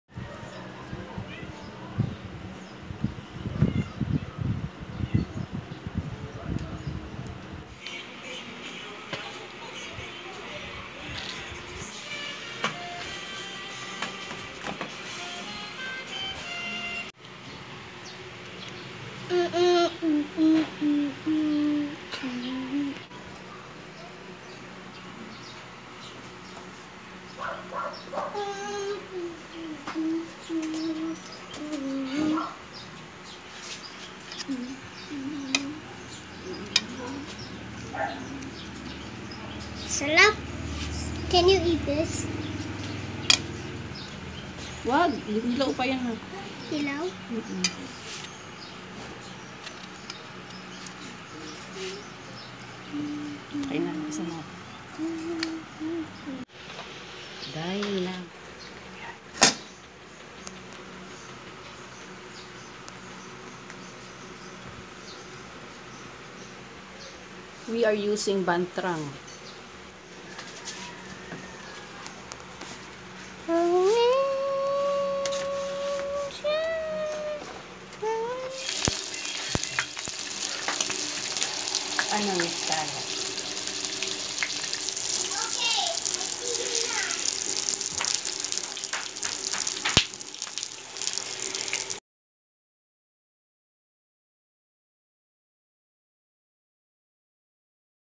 How to make Dynamite with Bahn Trang wrapper
So easy

This are the ingredients

Siling haba
Giniling
Sibuyas
Bawang
Carrots
Cheese
Bahn Trang wrapper
Cooking oil
Pwedi rin naman lumpia wrapper

Procedures

Hiwain ang sili at tanggalan ng buto
Igisa ang bawang,sibuyas ,carrots at pork
Hiwain ang cheese

Kapag naigisa na ang giniling pwesde na natin ipalaman sa sili na natanggalan natin ng buto ,at sabay lagay narin ng cheese

Magpainit ng pan
Pag mainit na ,iprito na natin ang sili na may giniling

Kaya sya tinawag na dynamite kasi sobrang sasabog ang mouth mo sa sobrabg anghang pero sobrang sarap naman talaga.